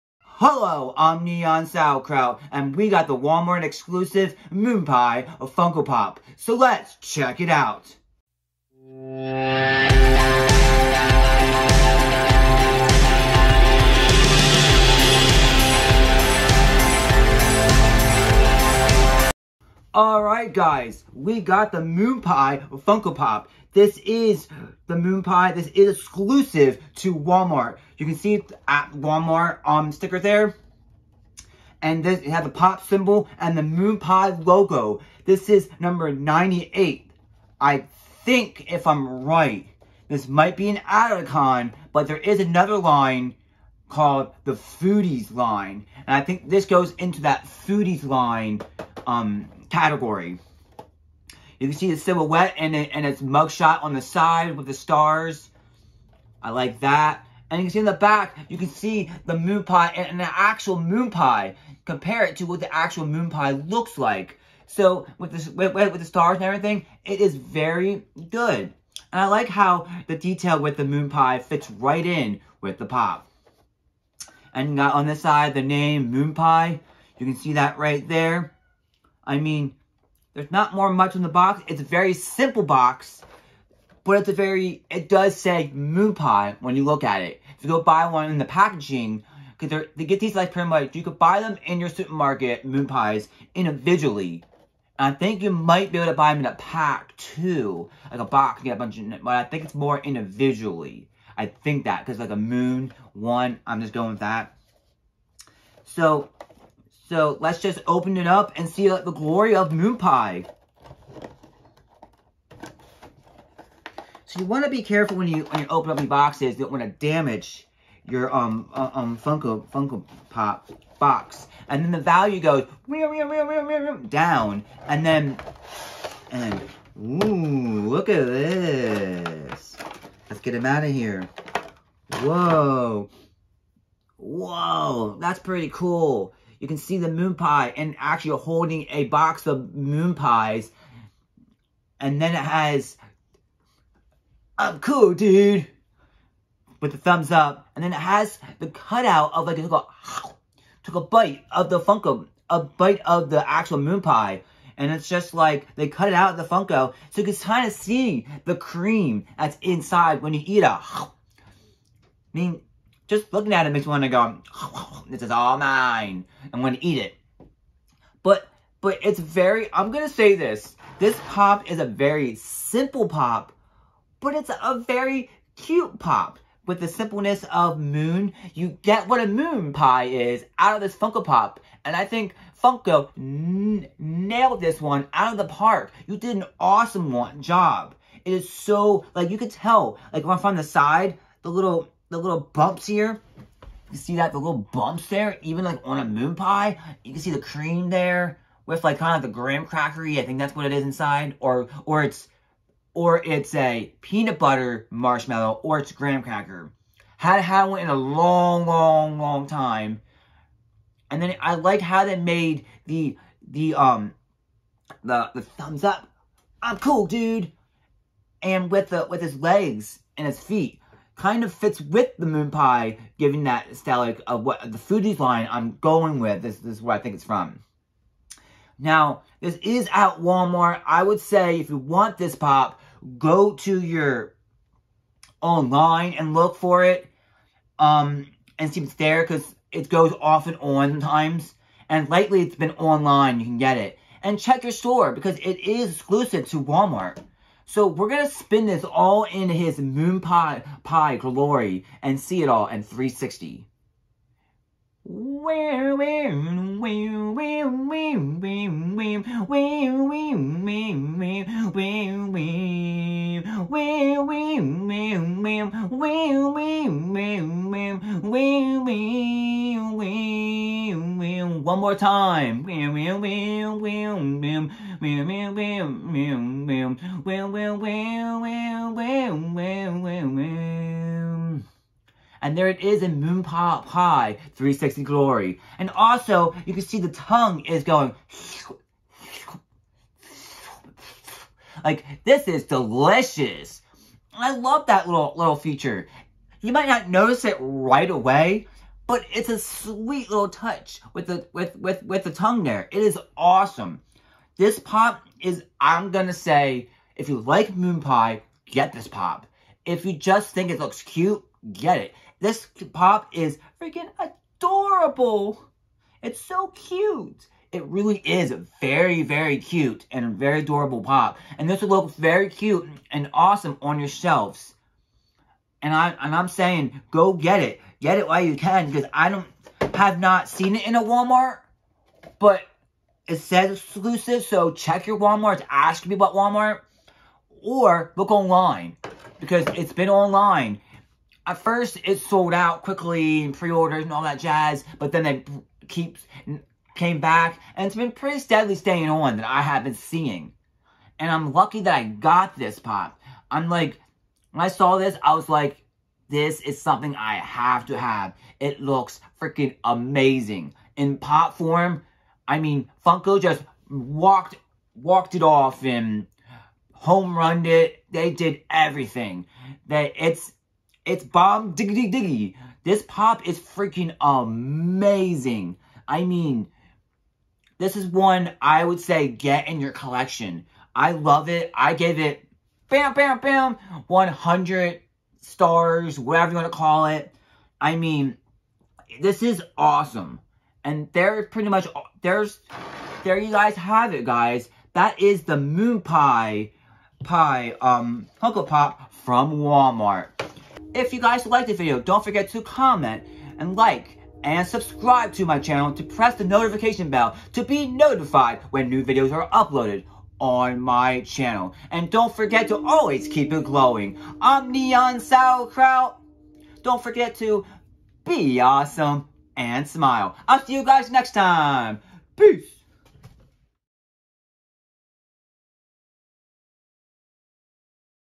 Walmart Exclusive Moon Pie Funko Pop Review
Come see a tasty snack that is turn into a funko pop moon pie as a funko pop come see as I review the moon pie funko pop come see if it is as tasty as moon pie snack is and see if you want to get to pick up this pop for your self